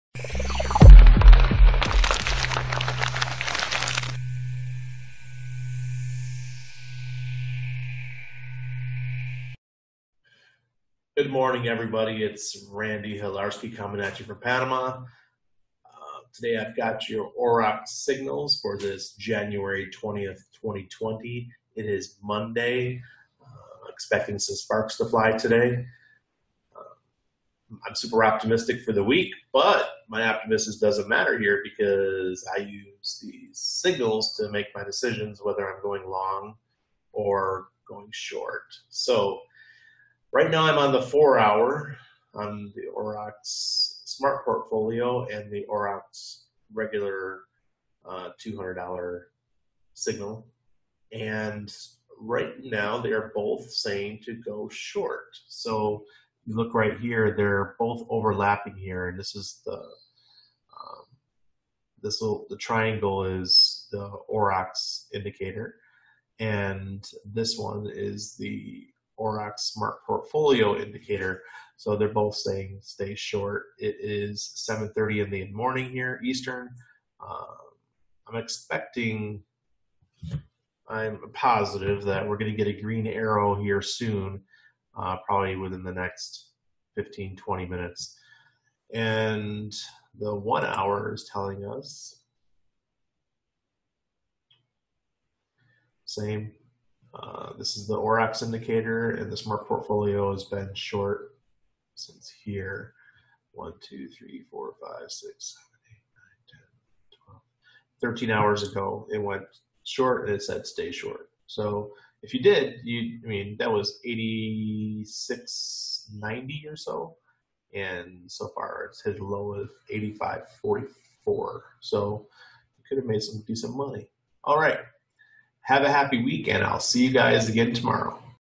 Aurox Bitcoin Trading with Indicators or Signals Number 4
My daily bitcoin trading video using Aurox trading indicators.